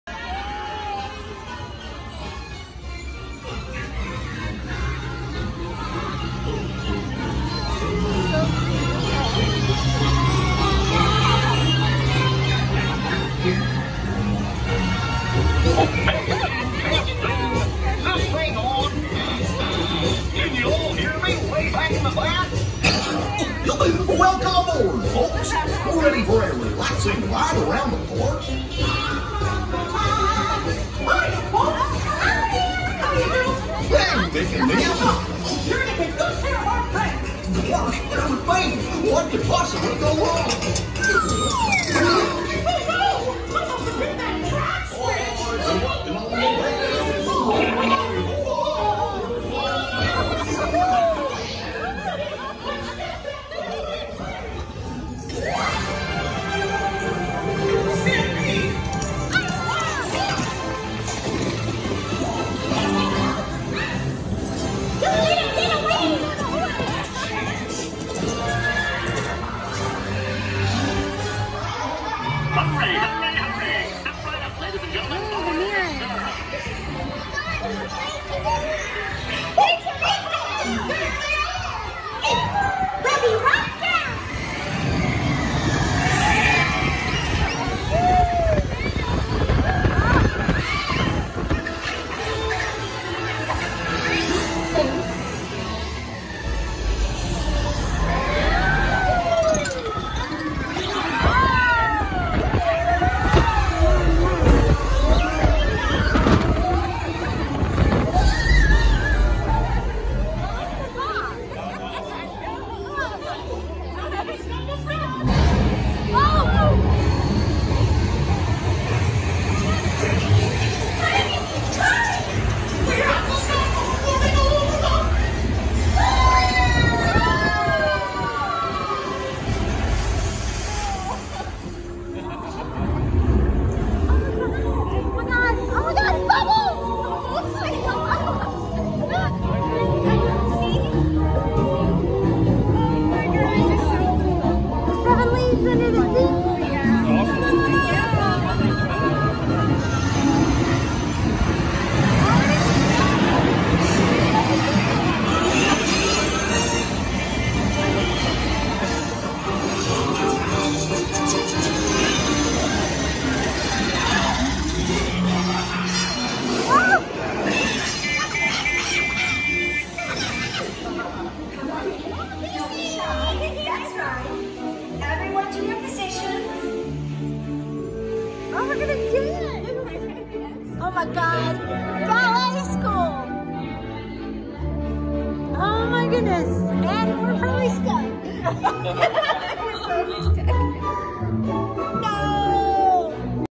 Mickey and Minnie’s Runaway Railway Part 2
We got stuck or Stopped for a little while like a minute.
Not Broke down. So that’s enough time for me to have a Nap ( in my Full video you could see)
This video is the part after the Dance Lesson with Daisy into the rest of the End of the Train ride with Goofy.
Hollywood Studios March 2020